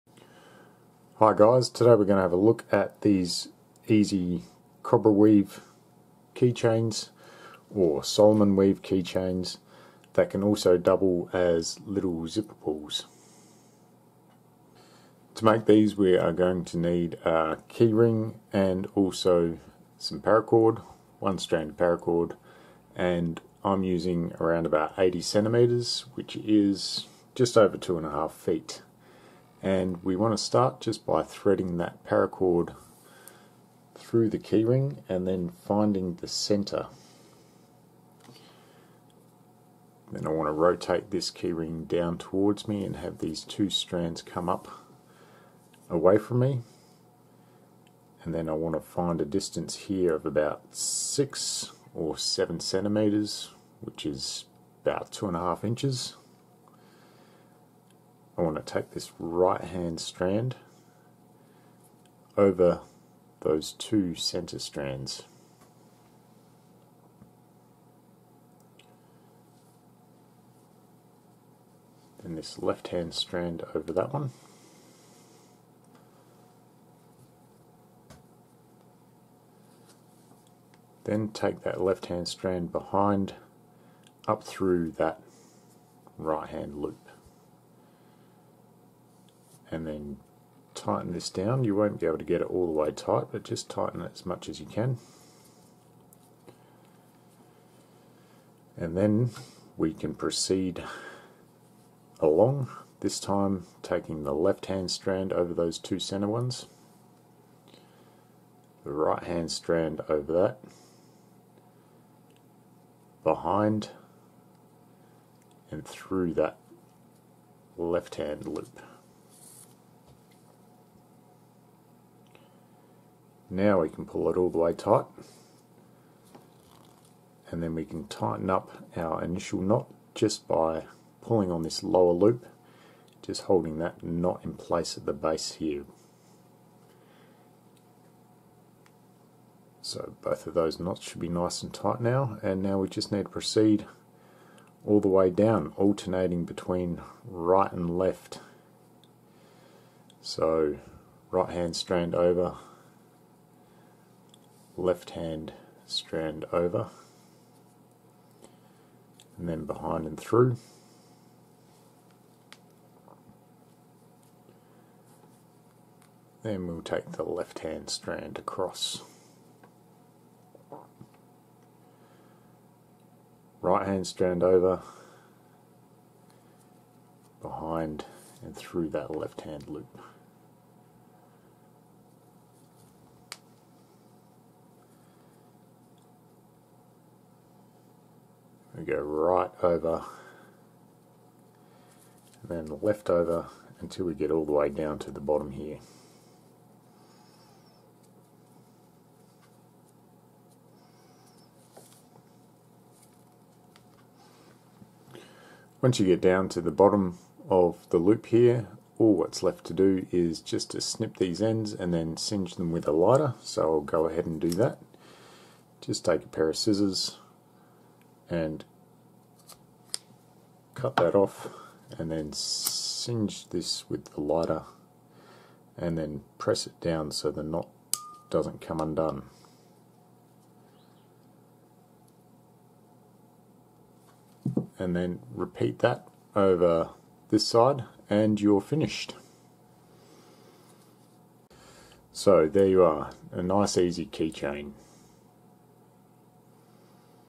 How To Make The Easiest Paracord Keychain - WhyKnot
Knot tying video tutorial. Learn how to make a homemade Solomon bar key fob. Easy step by step instructions in this guide.

Video demonstration on how to make a simple handmade key fob, using a cobra knot or cobra stitch. Makes a cool paracord project. Can also be used to make a zipper pull or ID lanyard.